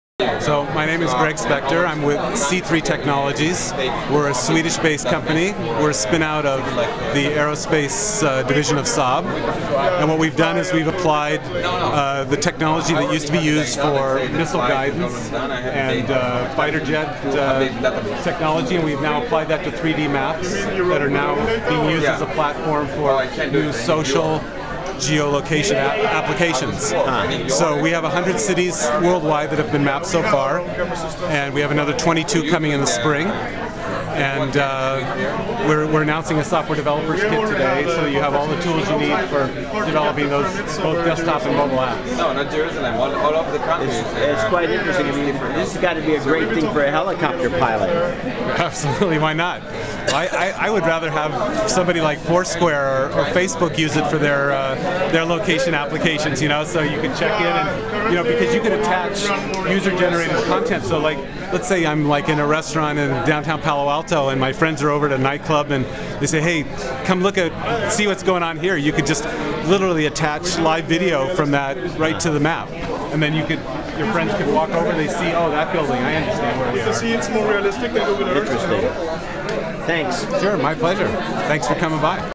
Wild 3D Mapping
A Swedish company does 3D mapping right!